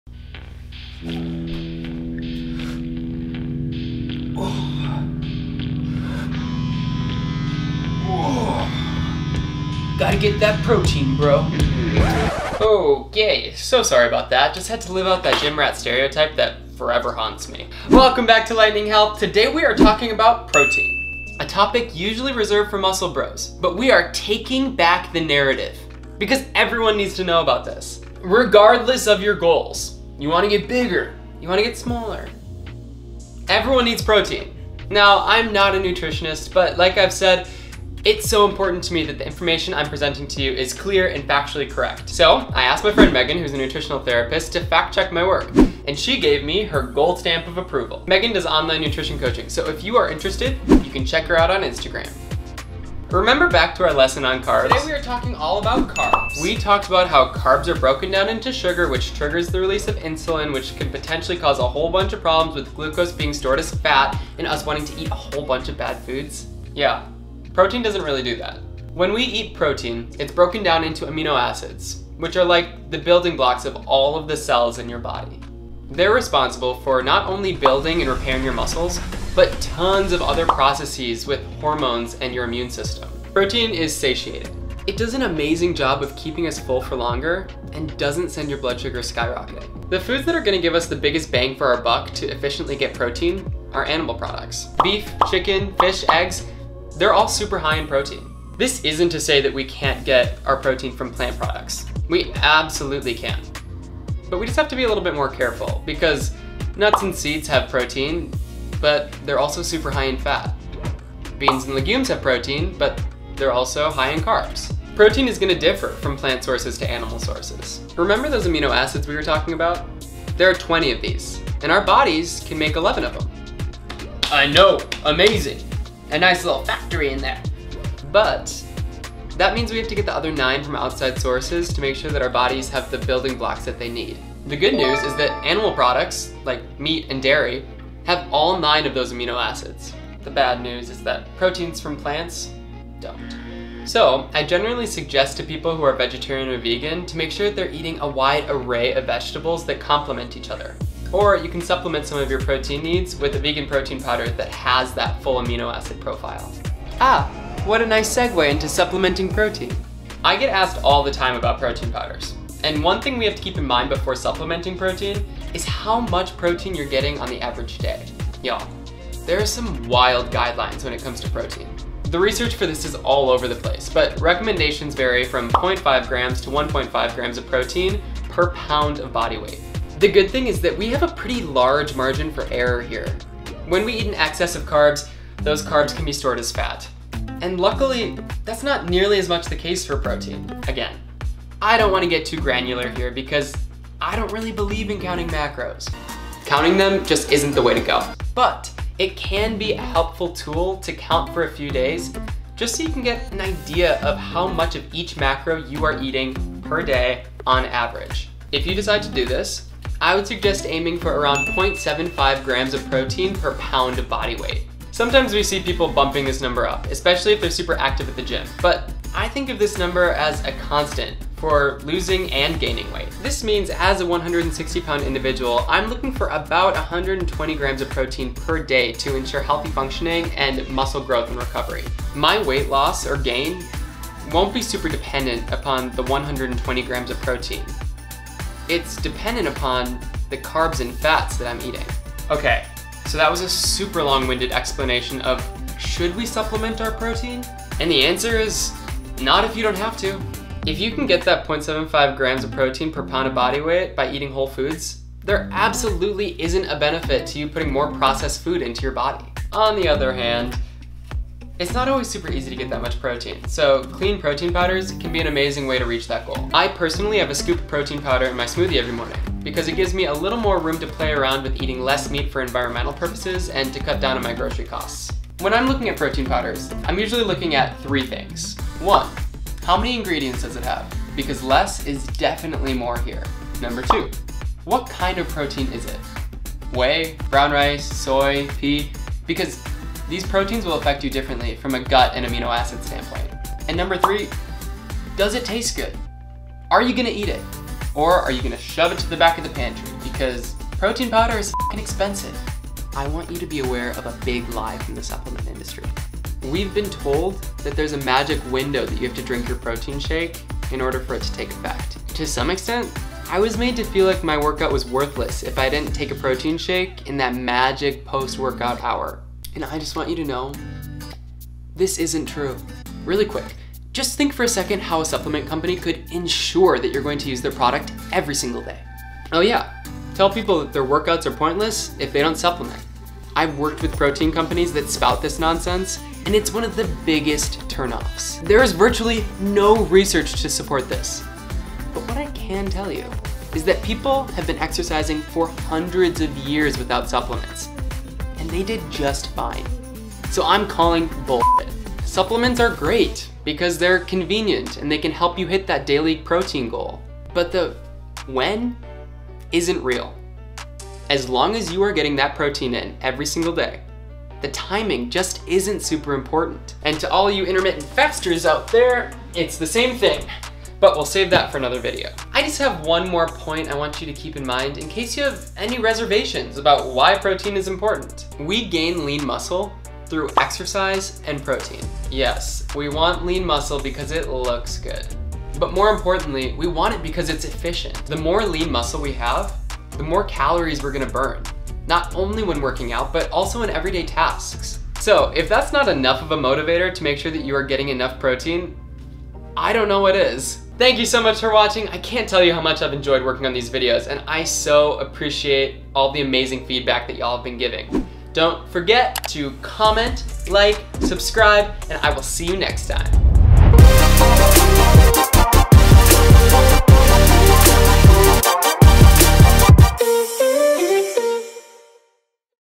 PROTEIN: NOT ONLY FOR BROS (What is Protein & Why Do We Need It 🥩) | PRIDEFIT
🥩 Today we're talking all about PROTEIN - what it does for us, who needs it, and WHY! Long story short, whether you're trying to lose, gain, or maintain weight, protein is a must. Just watch the video and you'll see.

🥩 Check back on Thursday for a new weighted lower body workout!